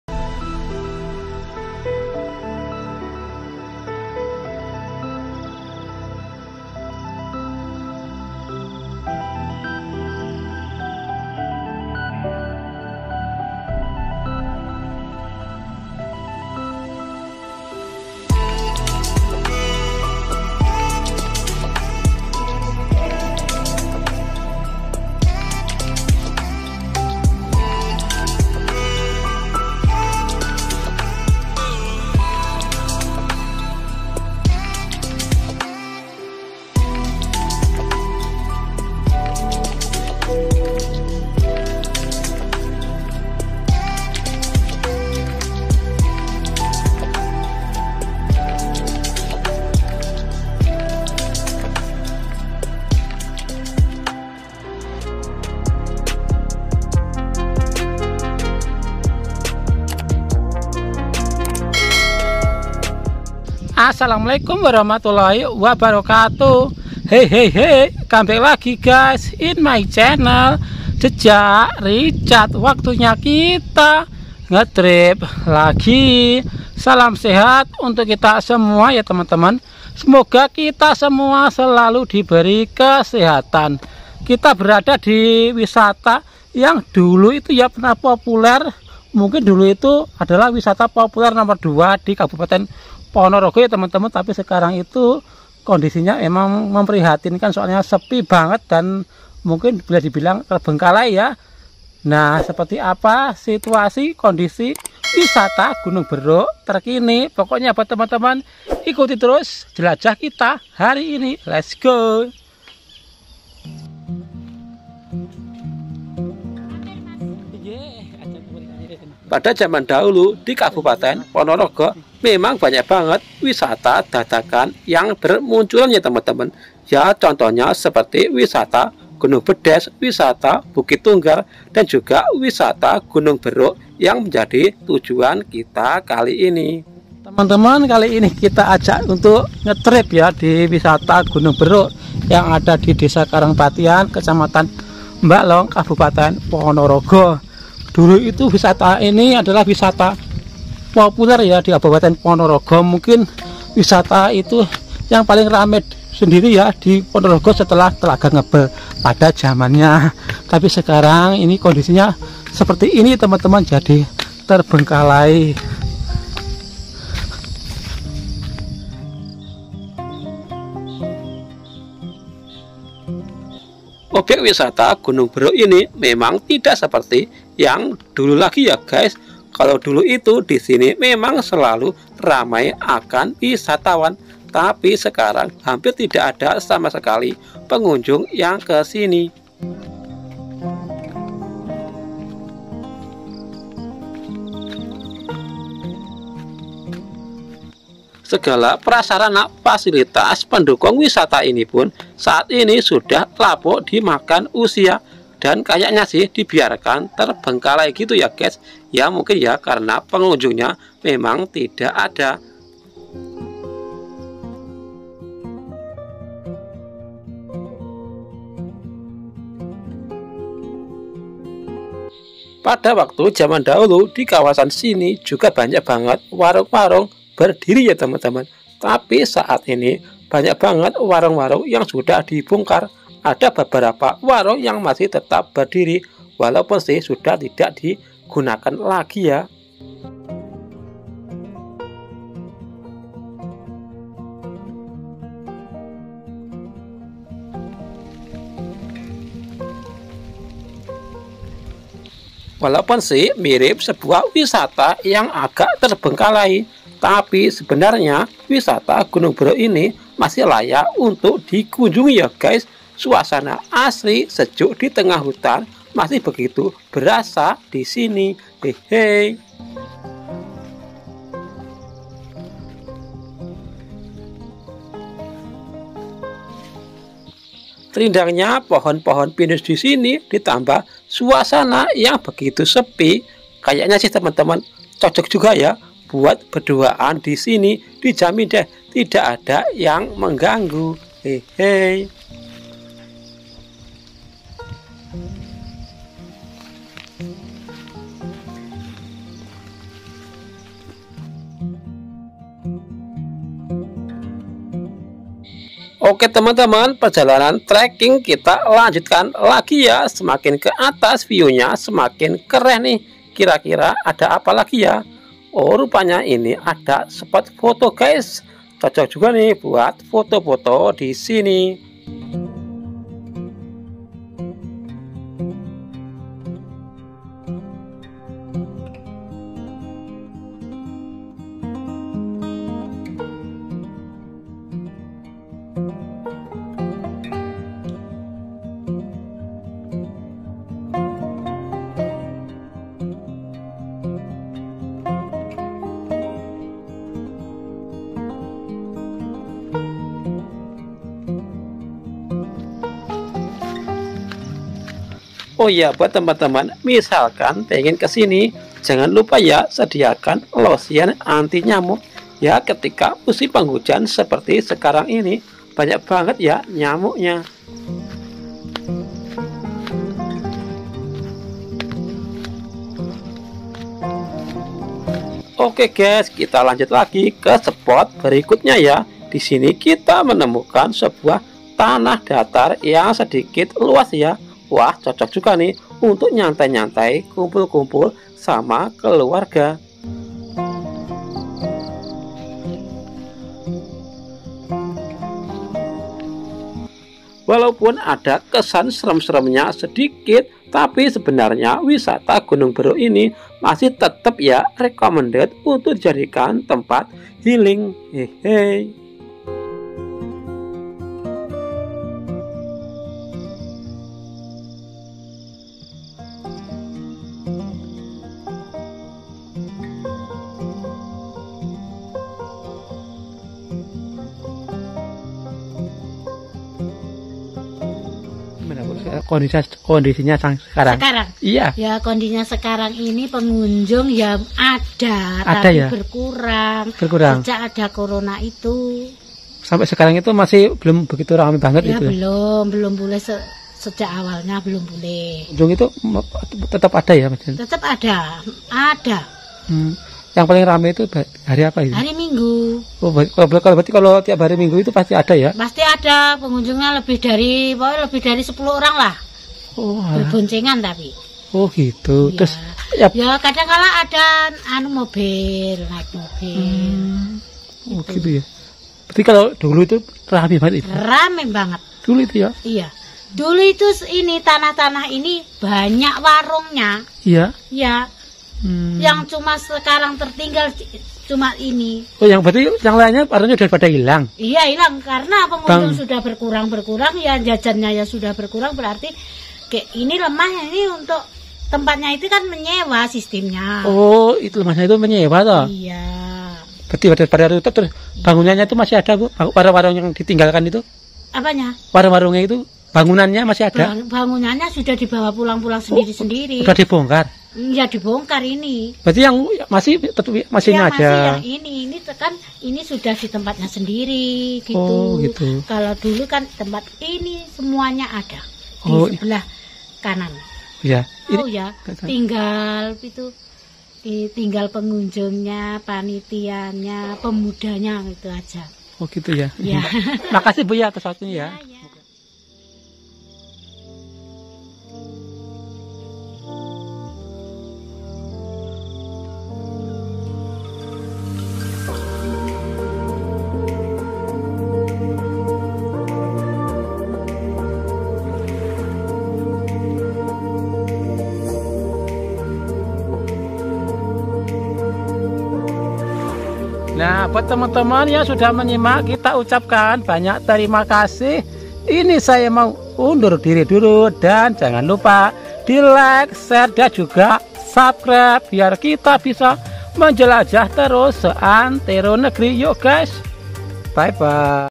Wisata Terbengkalai Tengah Hutan Pedesaan Ponorogo
wisata gunung beruk yg berada di desa karangpatihan Balong Ponorogo pernah berjaya di masanya dulu wisata ini sangat ramai dikunjungi wisatawan banyak warung berdiri di sini tapi sekarang kondisinya jadi sepi warung warung sudah tidak di gunakan lagi